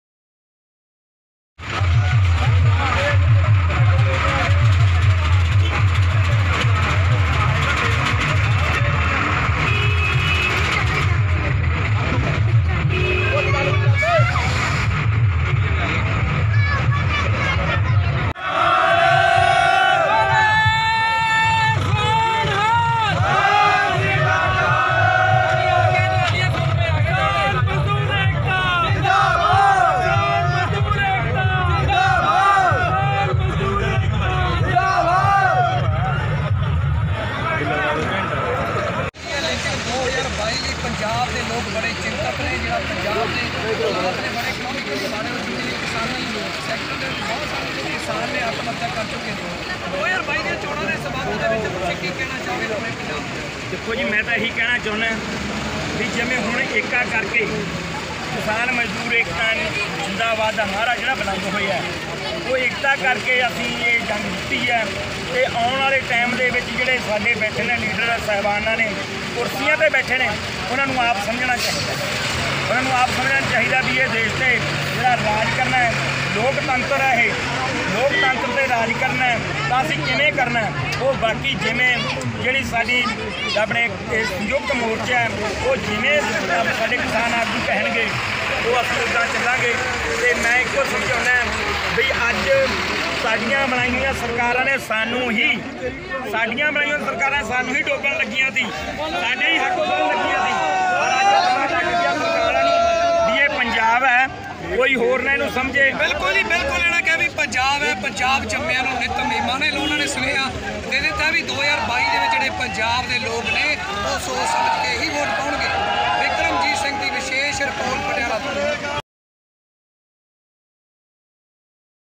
ਪੰਜਾਬ ਸਰਹੱਦ ਸ਼ੰਭੂ ਤੇ ਦਿੱਲੀ ਤੋ ਪਰਤ ਰਹੇ ਕਿਸਾਨਾ ਦਾ ਜ਼ੋਰਦਾਰ ਸਵਾਗਤ ।। ਕੀਤੀ ਜਾ ਰਹੀ ਹੈ ਫੁੱਲਾ ਨਾਲ ਵੱਰਖਾ
ਪੰਜਾਬ ਸਰਹੱਦ ਸ਼ੰਭੂ ਤੇ ਦਿੱਲੀ ਤੋ ਪਰਤ ਰਹੇ ਕਿਸਾਨਾ ਦਾ ਜ਼ੋਰਦਾਰ ਸਵਾਗਤ ।। ਕੀਤੀ ਜਾ ਰਹੀ ਹੈ ਫੁੱਲਾ ਨਾਲ ਵੱਰਖਾ ।। ਬਹੁੱਤ ਹੀ ਮਨ ਮੋਹਕ ਨਜਾਰਾ